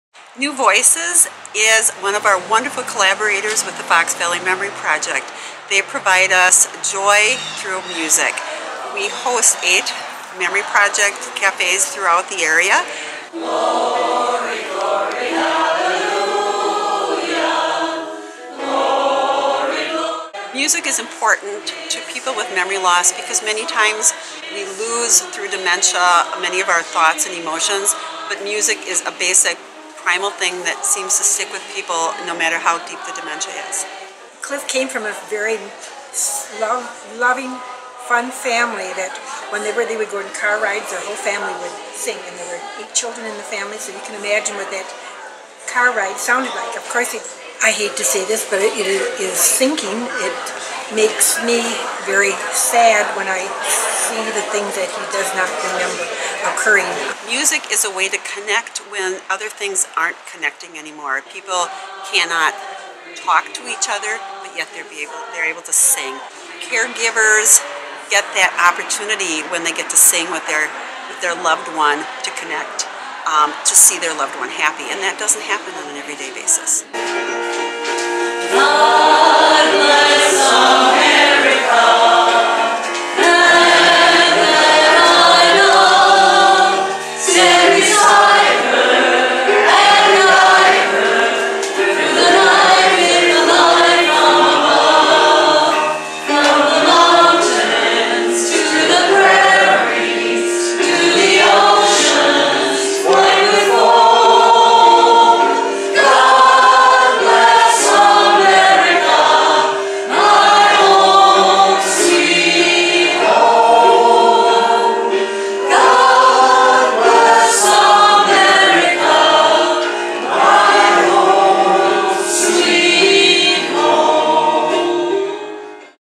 newVoices brings music to Memory Cafes
As part of our ongoing community service, newVoices collaborates with the Fox Valley Memory Project to lead singing at area Memory Cafes. Long after memories fade, music remains for those with memory loss. Through this program, our volunteer singers take music out of the concert hall to those who need it most. and for more information on Fox Valley Memory Cafes,